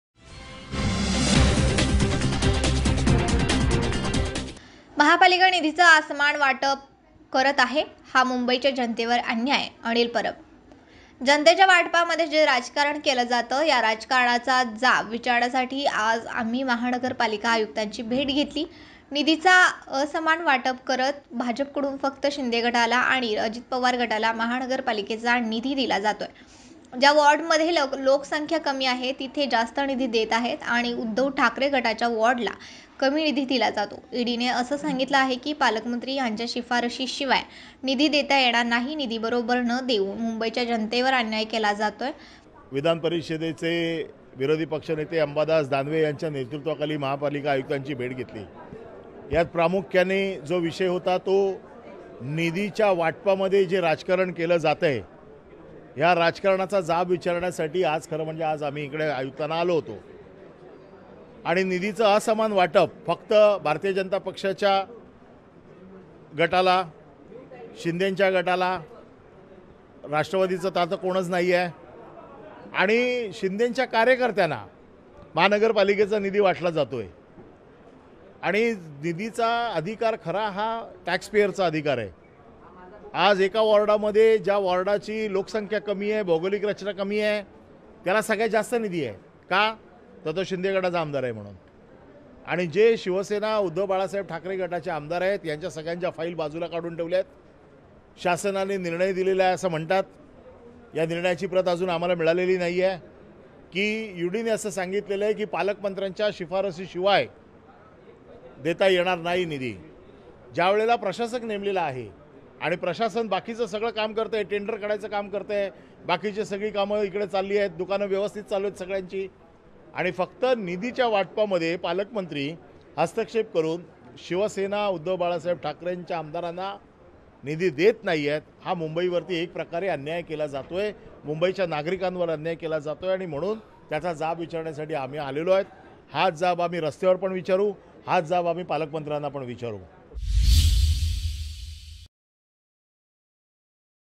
MUMBAI | महापालिका निधीचं असमान वाटप करत आहे, हा मुंबईच्या जनतेवर अन्याय - अनिल परब
SBN MARATHI
महापालिका निधीचं असमान वाटप करत आहे, हा मुंबईच्या जनतेवर अन्याय - अनिल परब